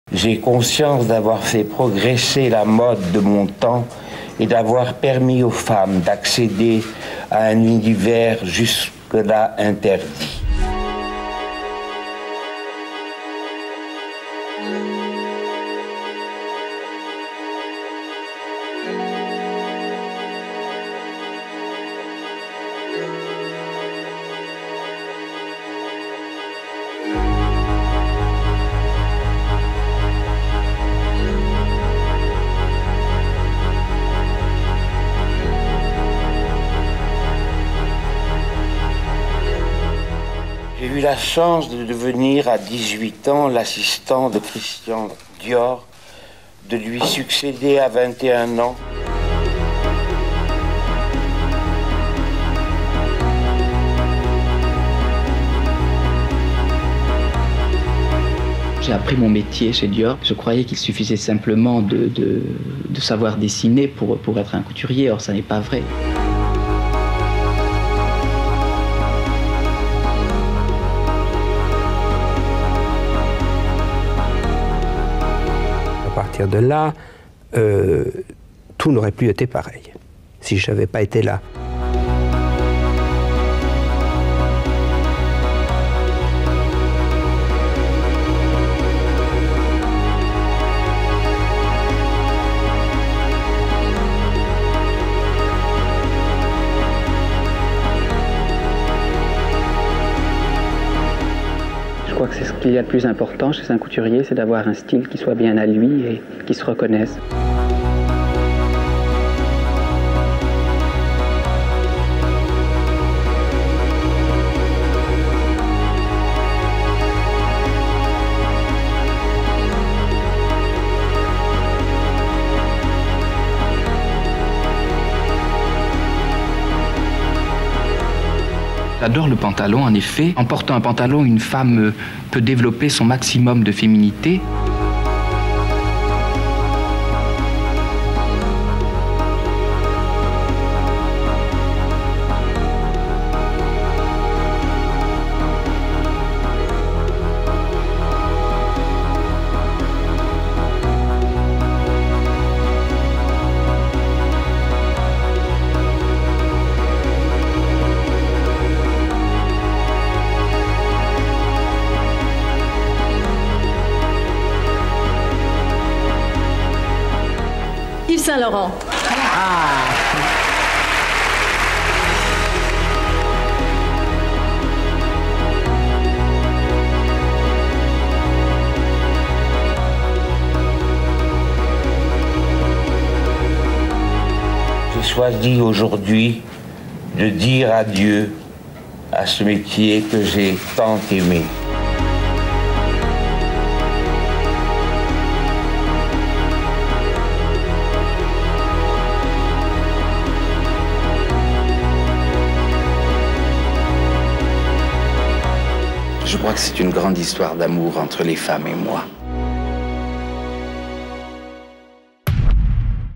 Yves Saint Laurent
Il a révolutionné la mode en libérant le corps des femmes. Yves Saint Laurent est décédé le 1 juin 2008, c’était il y a 10 ans. Voici son histoire.